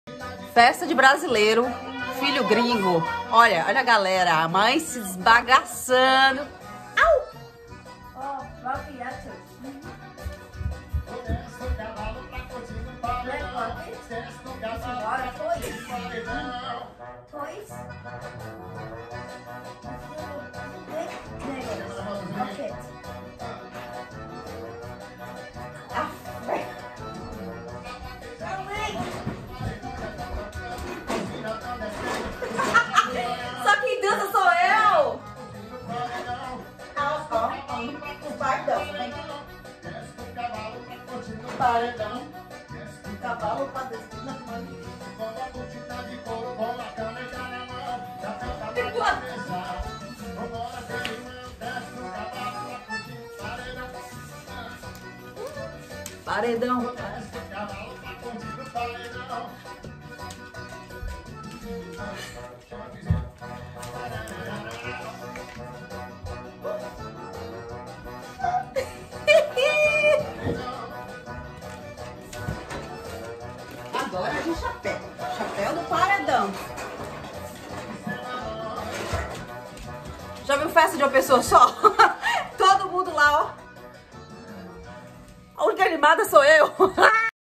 Mãe Brasileira filho gringo
Moms brasileiras,vocês sofrem desse mal?Convido as pra formarmos um grupo,grupo das mães de gringos ;)O que acham??? Mesmo com
Toda animação nos gens herdado da mãe ,típica do meu país tropical  ,meus filhos saíram pacatos cidadãos!!E sempre assim , eu me esbagaçando e eles balançando a cabeça 🤓Fala  sério!!🤦‍♀️🤦‍♀️🤦‍♀️